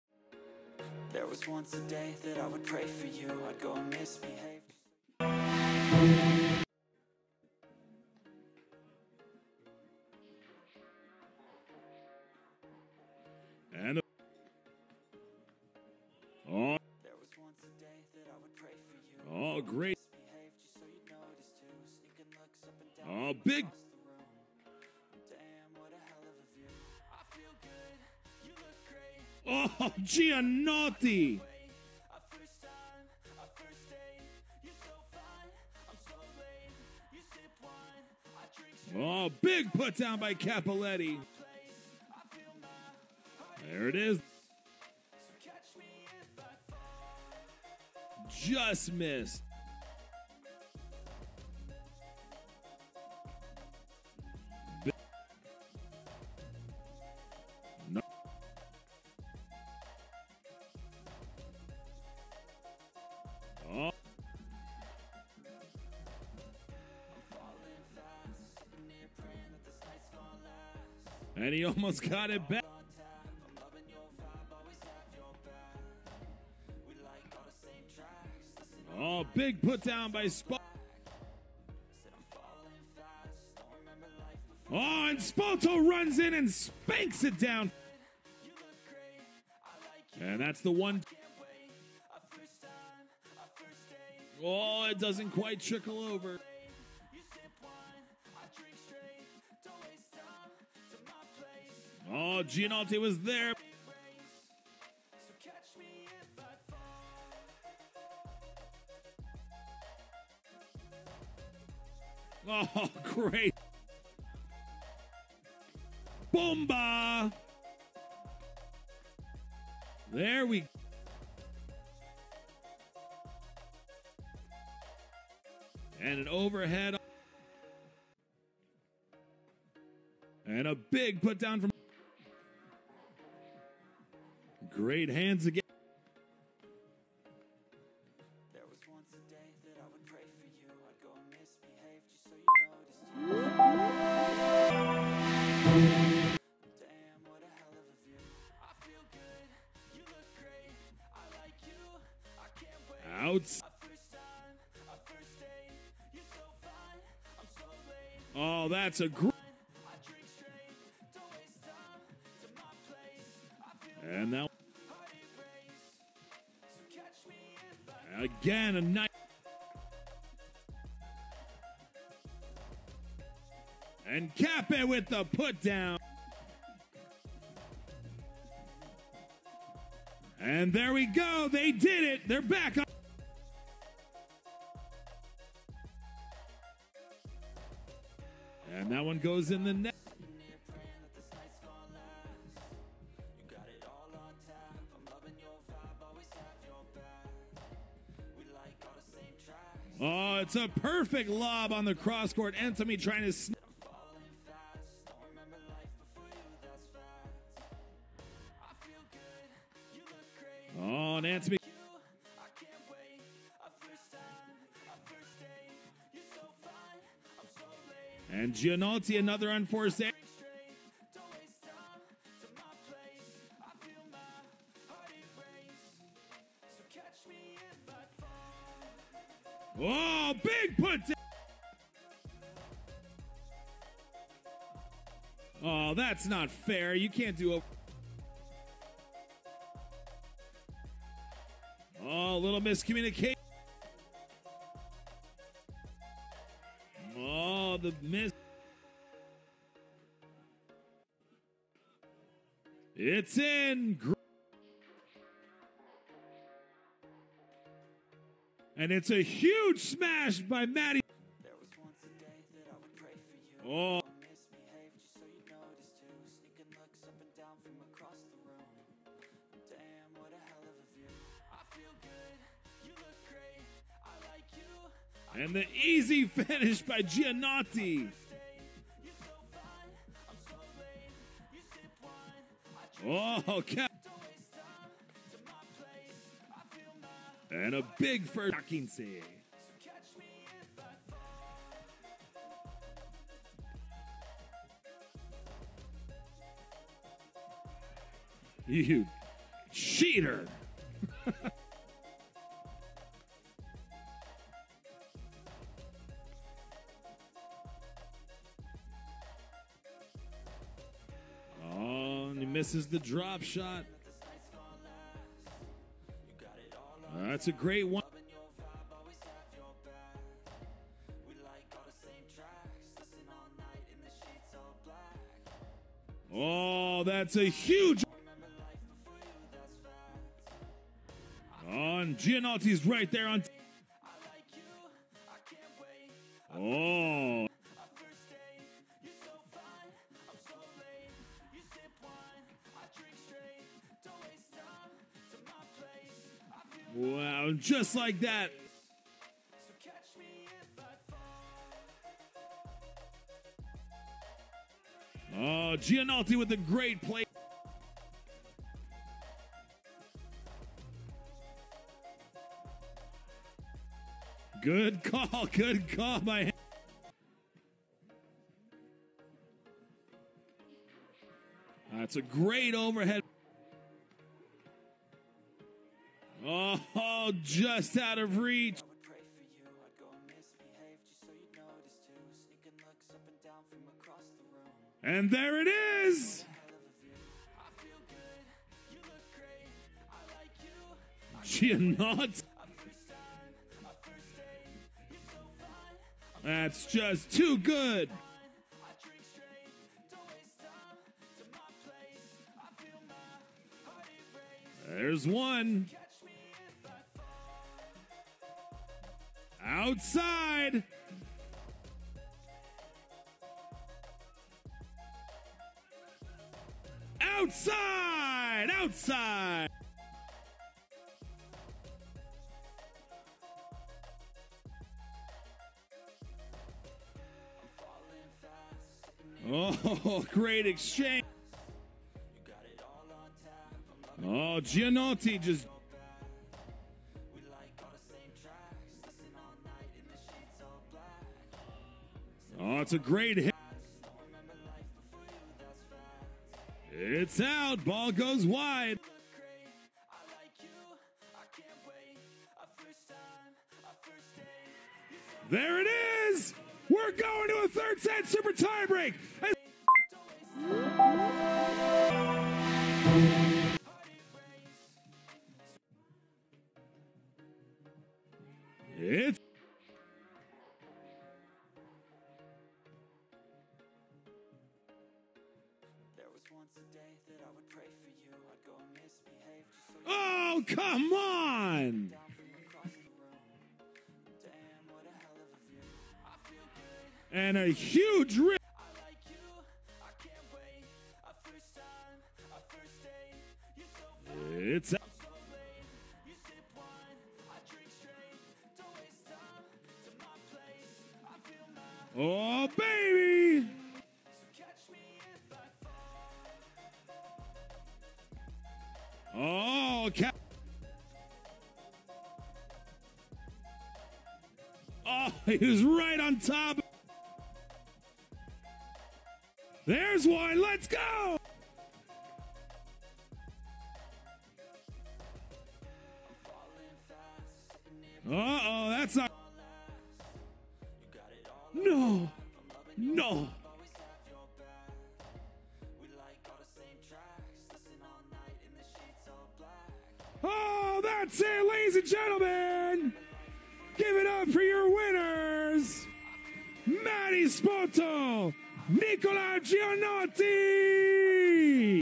Beach Tennis | Final Masculino | SAND SERIES PÄRNU 2023 - Melhores Momentos
🔥 Se gostou desse jogo, recomendo assistir esse aqui pois é melhor ainda ➡️